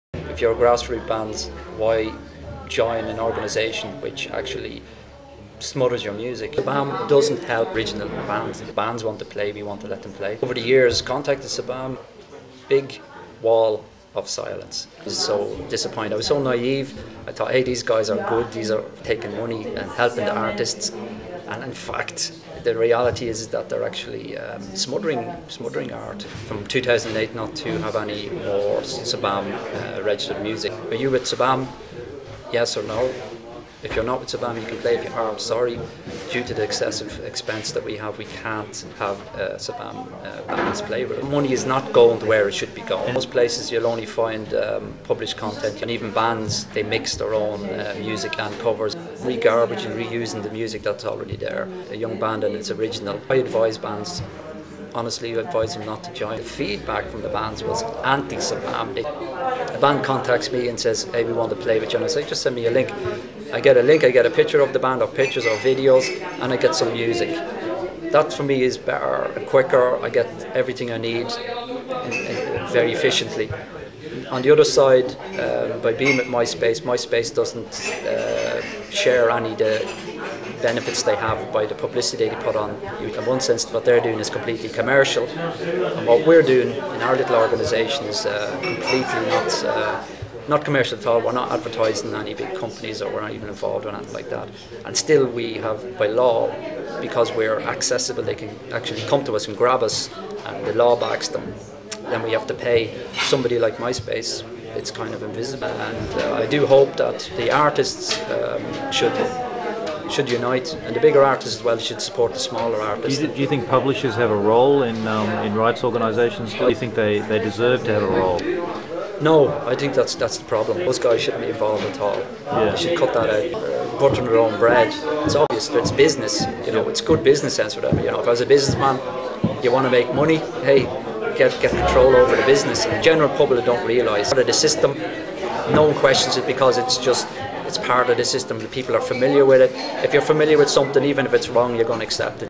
James King from sunshine concerts
Talking about the coruption of SABAM. Sabam help the large corporations, thats the way it is.